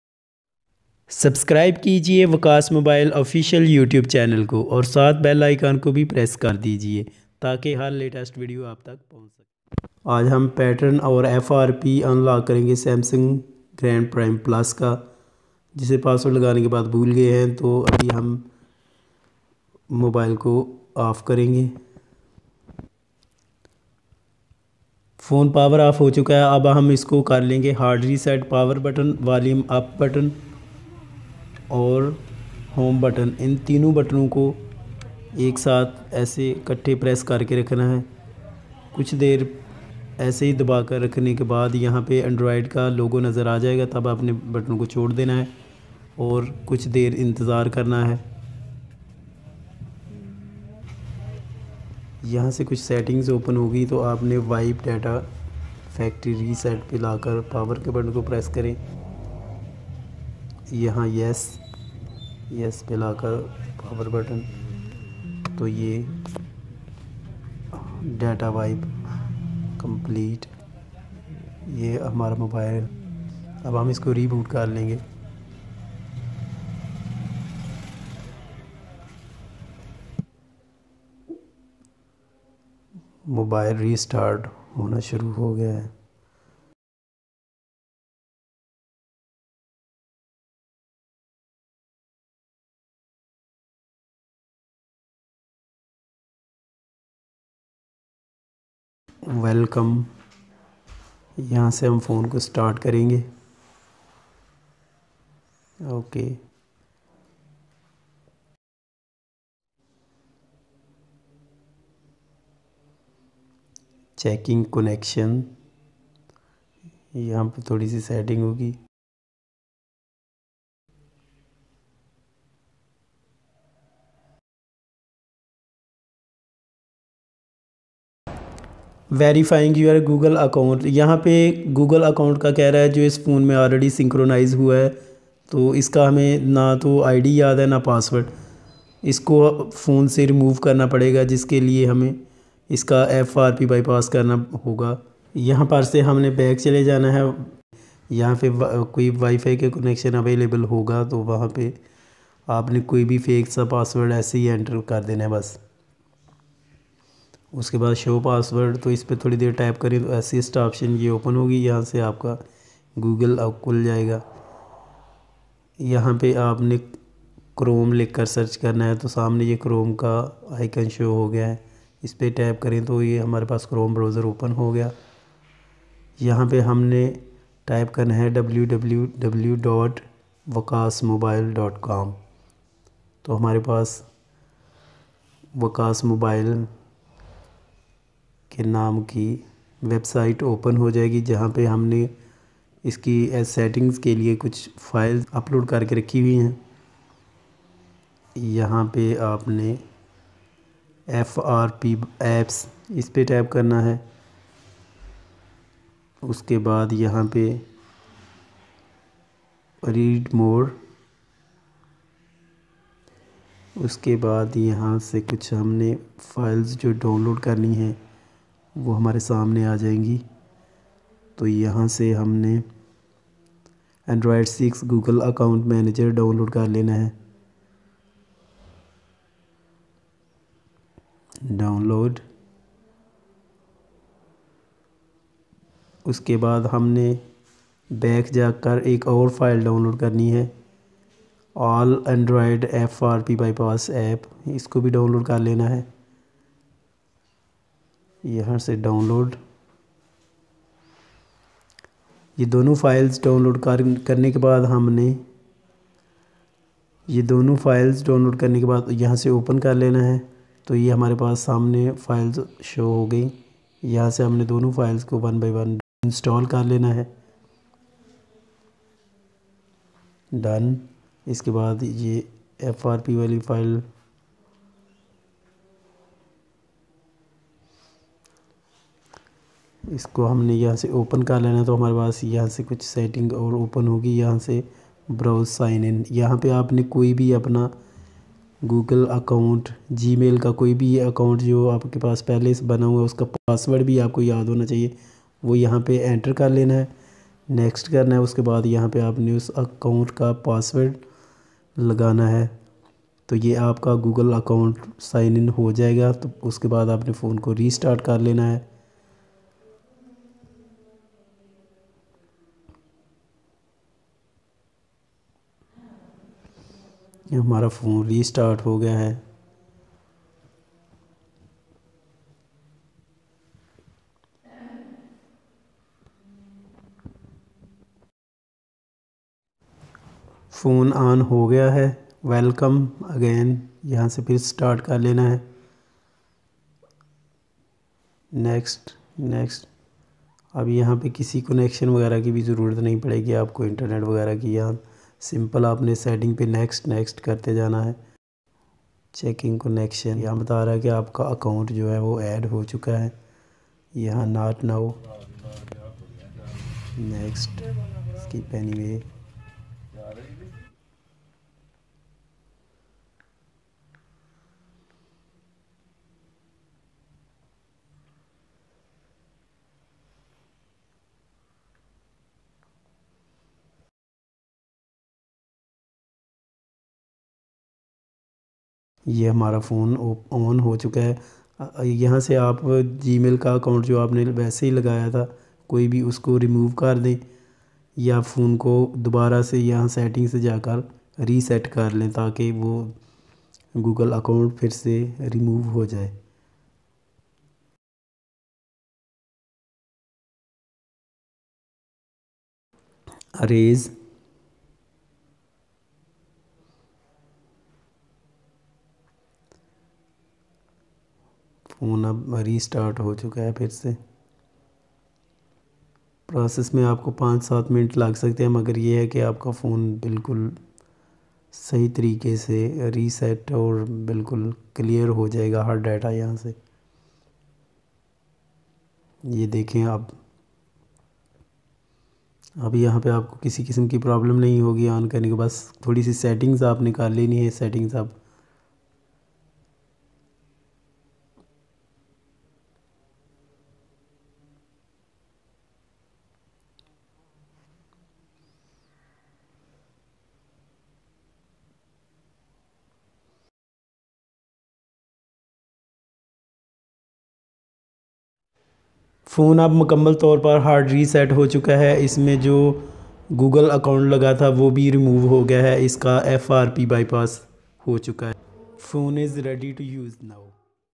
Frp bypass Samsung Grand prime+||pattern unlock Samsung Grand prime+
in this video you will learn how to remove pattern and Frp lock of Samsung galaxy Grand prime +,